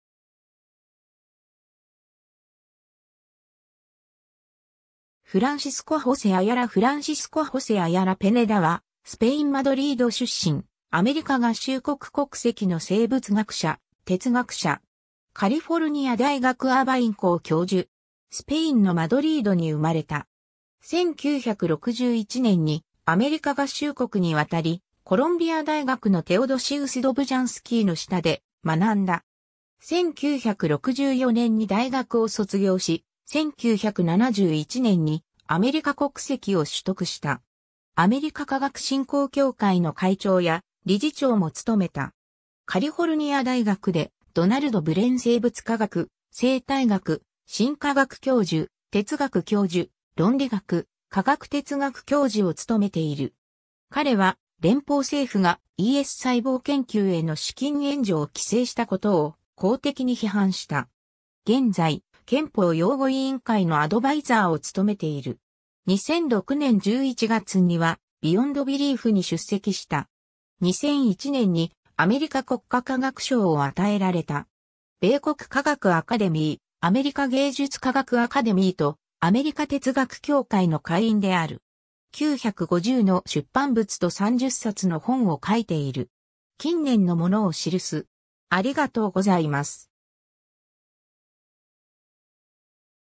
フランシスコ・J・アヤラ
フランシスコ・ホセ・アヤラ フランシスコ・ホセ・アヤラ・ペネダ（Francisco José Ayala Peneda, 1934年3月12日 - ）は、スペイン・マドリード出身、アメリカ合衆国国籍の生物学者、哲学者。
カリフォルニア大学アーバイン校教授。
 スペインのマドリードに生まれた。
1961年にアメリカ合衆国にわたり、コロンビア大学のテオドシウス・ドブジャンスキーの下で学んだ。
1964年に大学を卒業し、1971年にアメリカ国籍を取得した。
アメリカ科学振興協会の会長や理事長も務めた。
 カリフォルニア大学でドナルド・ブレン生物科学・生態学・進化学教授、哲学教授、論理学・科学哲学教授を務めている。
彼は連邦政府がES細胞研究への資金援助を規制したことを公的に批判した。
現在、憲法擁護委員会（議会にES細胞研究への資金援助規制を解除するよう働きかけた）のアドバイザーを務めている。
2006年11月にはBeyond Belief（科学と宗教の関わりについて討論するシンポジウム）に出席した。
 2001年にアメリカ国家科学賞を与えられた。
米国科学アカデミー、アメリカ芸術科学アカデミーとアメリカ哲学協会の会員である。
 950の出版物と30冊の本を書いている。
近年のものを記す。